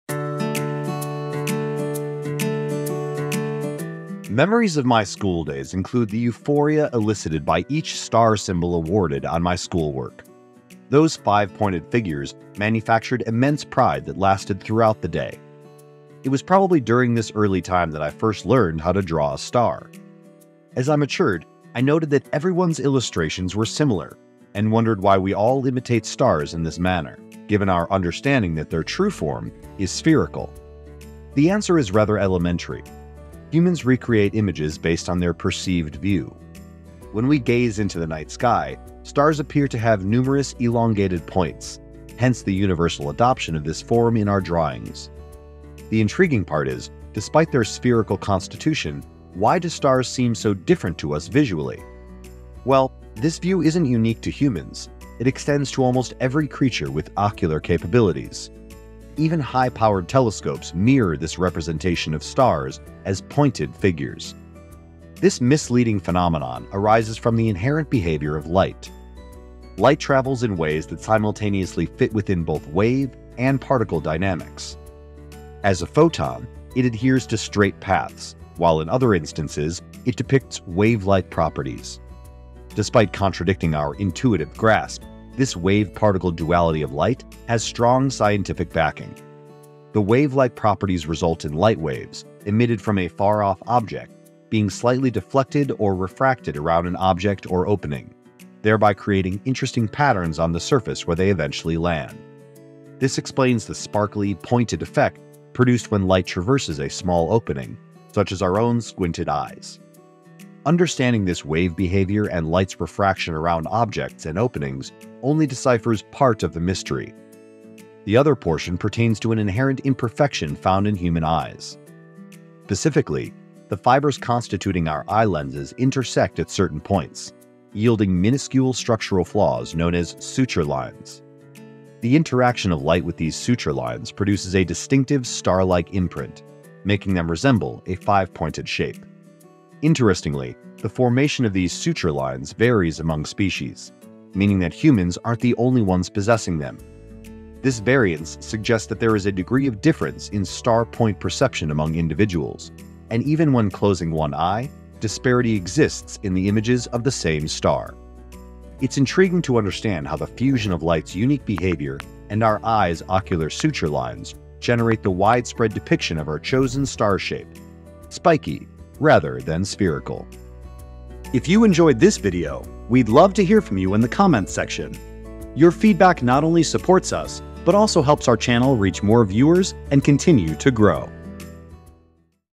Why Do Stars Seem To Have 5 Points (Corners)?
When light emitted from a distant object reaches another object or opening, its waves are bounced or bent slightly around the object and interfere with each other to produce various patterns on whatever they ultimately fall on. For example, light passing through a slab with a tiny slit leaves its imprint in the form of a perpendicular series of dashes. This is the reason why any light source appears to sparkle with pointed corners when you squint your eyes.

However, this only covers one part of the explanation. The other part has to do with our eyes, or rather, certain imperfections in our eyes.

The fibers that make up our eye lenses meet at certain points, resulting in very small structural imperfections called suture lines. Light, upon passing through the lens, interacts with these lines and consequently leaves a very distinct impression of a star, making it look like a pointy, five-armed toy.

Stock Image Source: elements.envato.com , pexels.com , pixabay.com, freepik.com, Wikimedia Commons